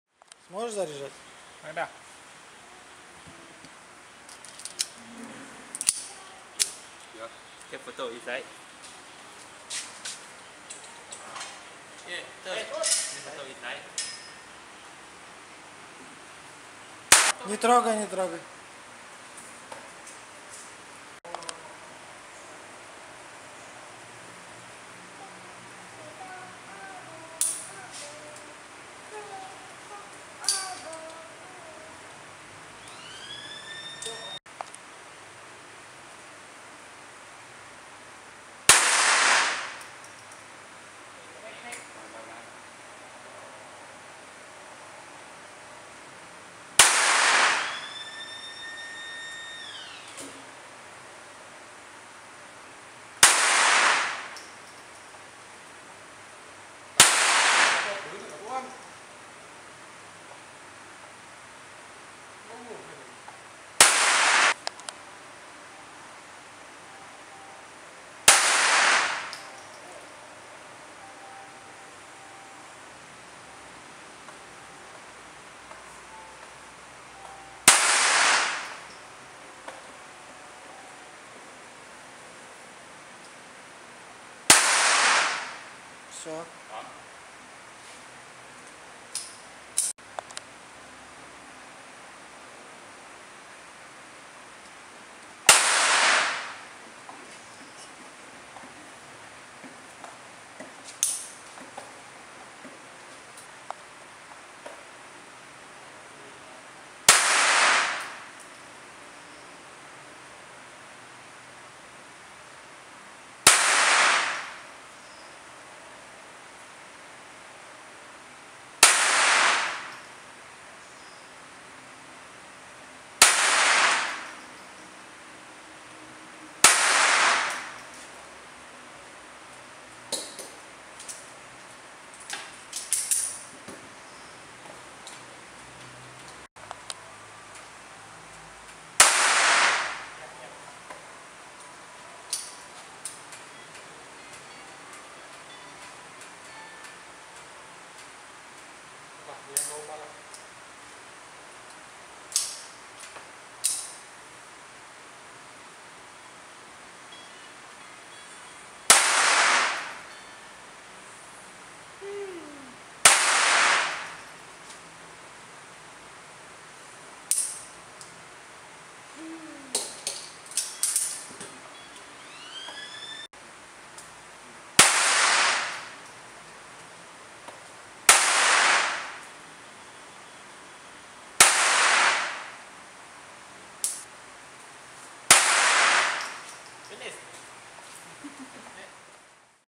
Walther PPK 22LR Thanva Grigorev
My son first gun shooting experience